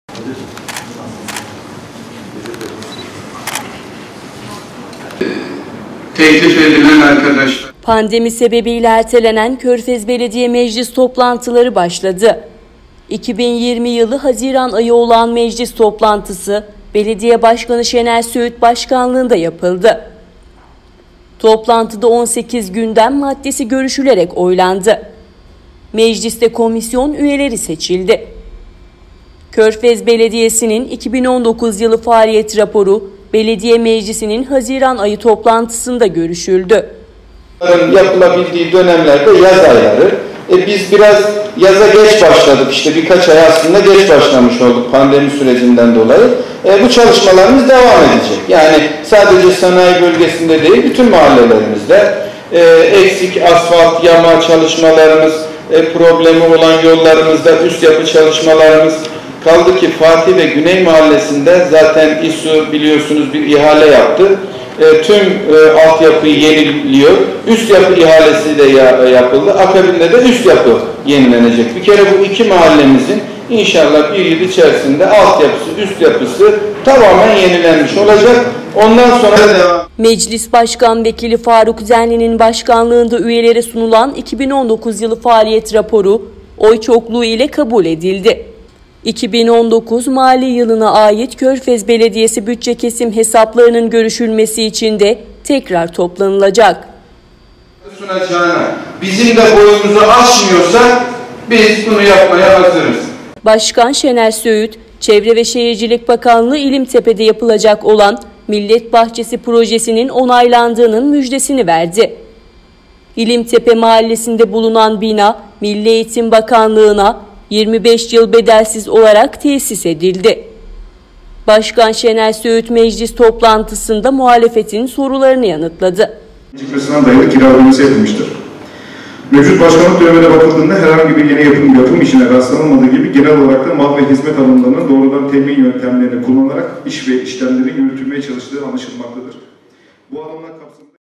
Körfez’de Pandemi’den Sonra İlk Meclis
Pandemi sebebiyle ertelenen Körfez Belediye Meclis toplantıları, 2020 yılı Haziran ayı olağan meclis toplantısıyla Belediye Başkanımız Şener Söğüt başkanlığında gerçekleştirildi. Tütünçiftlik Kültür Merkezi Kongre Salonu’nda yapılan toplantı açılış, yoklama ve saygı duruşu ile başladı. Toplantıda 18 gündem maddesi görüşülerek oylandı